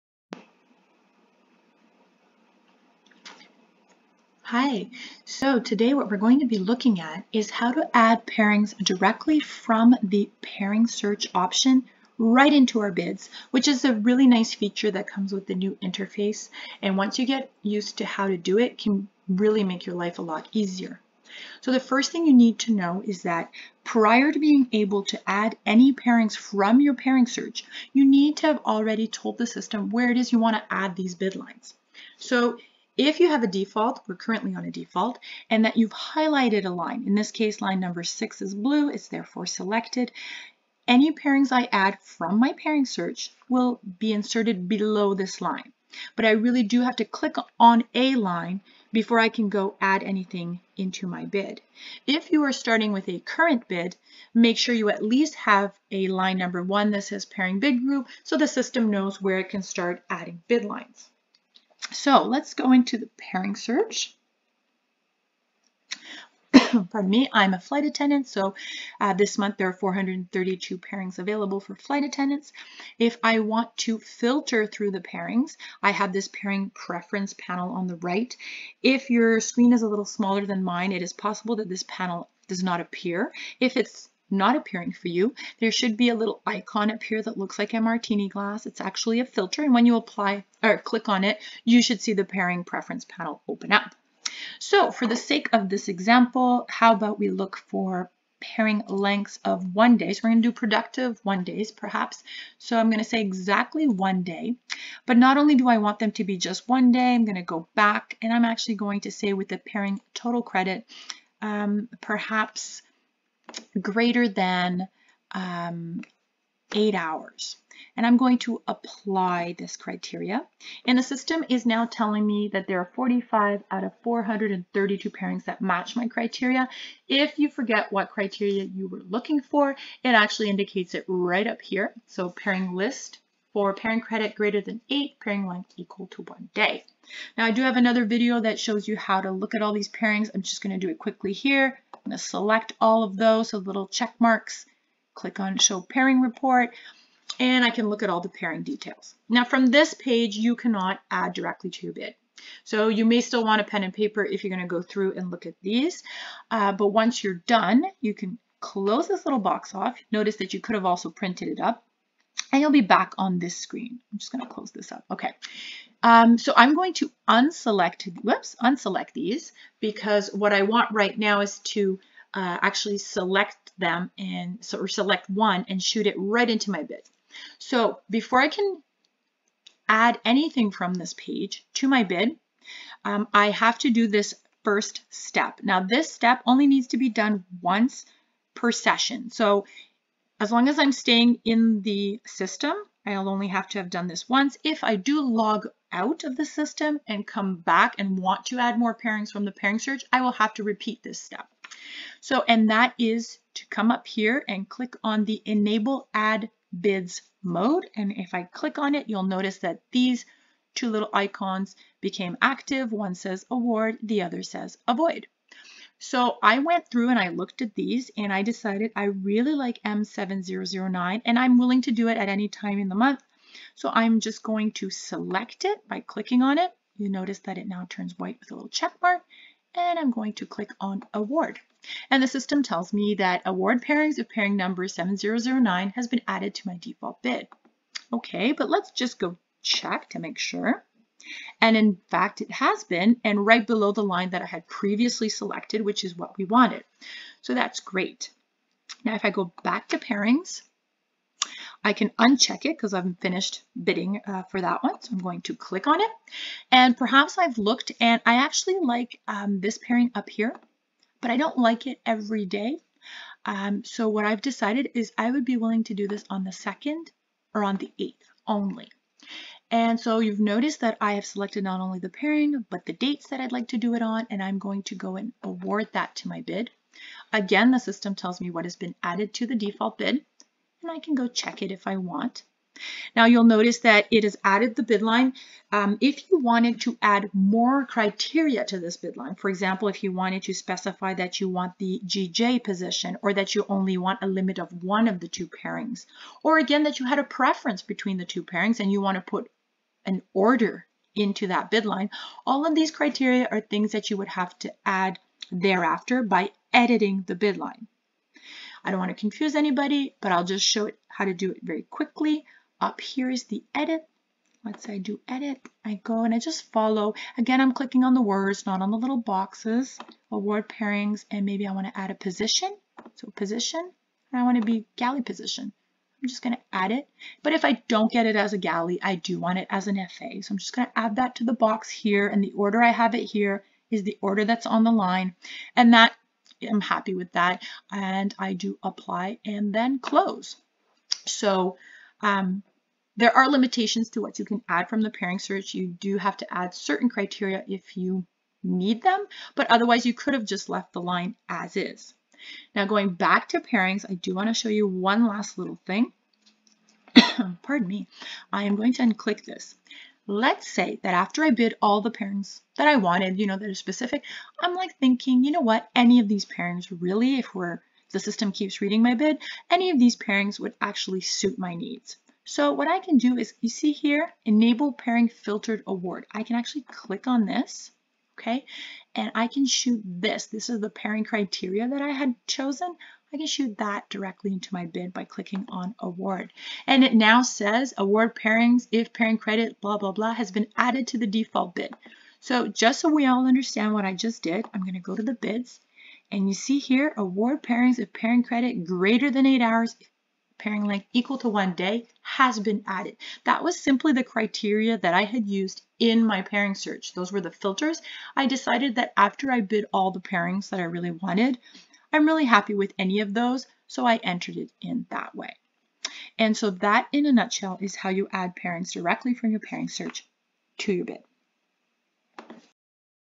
Add bid lines directly from pairing search
You can add pairings directly from the Pairing Search option.